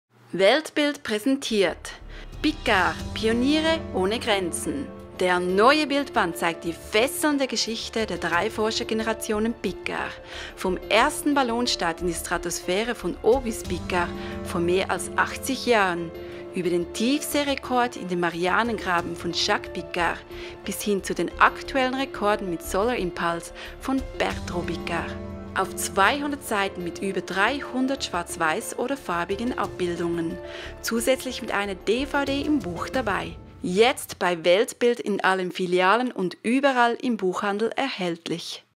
Piccard - Pioniere ohne Grenzen
Weltbild präsentiert: Piccard - Pioniere ohne Grenzen !

Die erste Bildbiographie über die Schweizer Forscher-Dynastie jetzt bei Weltbild und überall im Buchhandel erhältlich. Exklusiv bei Weltbild mit Bonus-DVD!